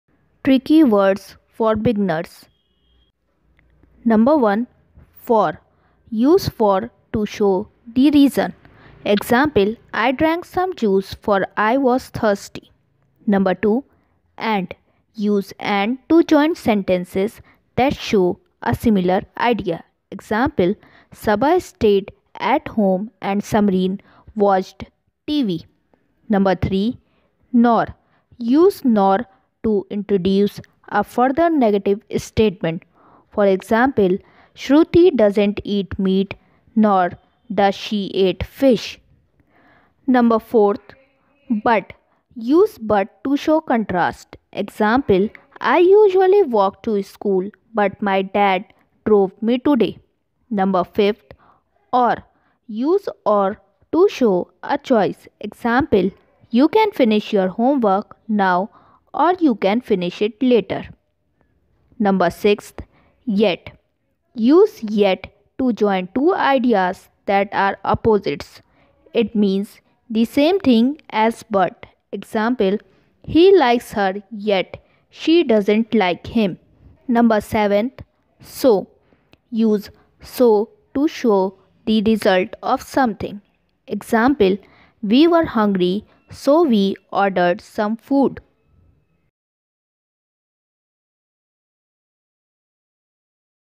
FANBOYS:Coordinating conjunctions
Tricky words for beginners.

Use for to show the reason.

Use and to join sentences that show a similar idea.

Use nor to introduce a further negative statement.

Use but to show contrast.

Use or to show a choice.

Use yet to join two ideas that are opposites. ( It means the same thing as but )

Use so to show the result of something.